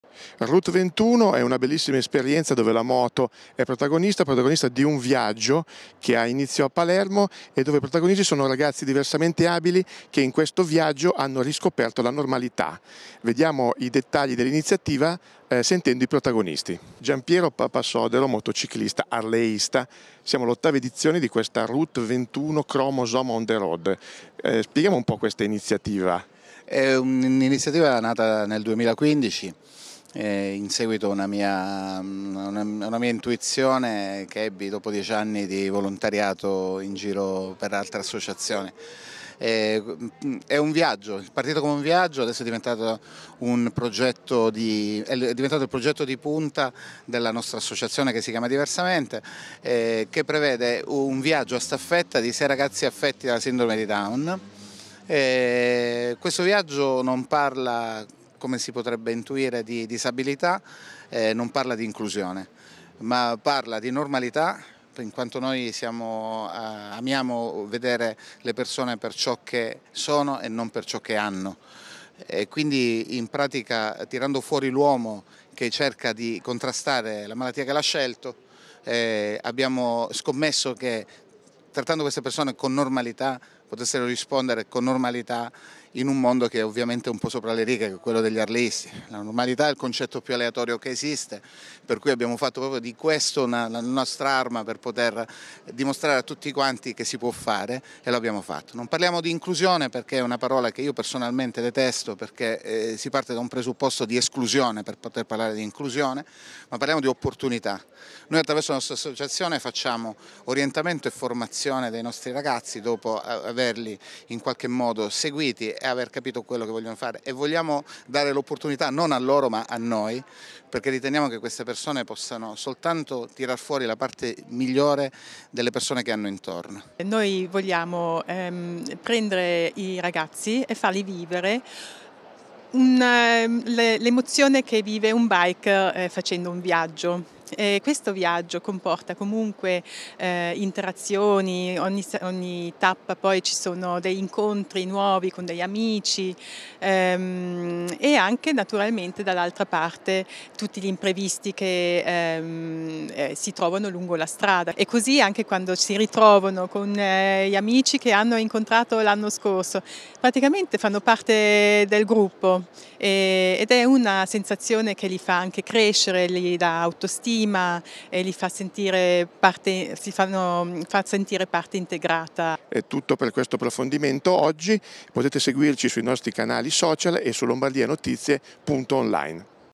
Tappa a Milano per 'Route 21 Chromosome on te road'
Il presidente di Regione Lombardia ha ricevuto oggi i protagonisti di 'Route 21 Chromosome on the road', un appassionante giro d'Italia dalla Sicilia al Veneto che Gian Piero Papasodero, biker harleysta impegnato nel sociale, sta realizzando accompagnando in diverse tappe sei ragazzi 'speciali'.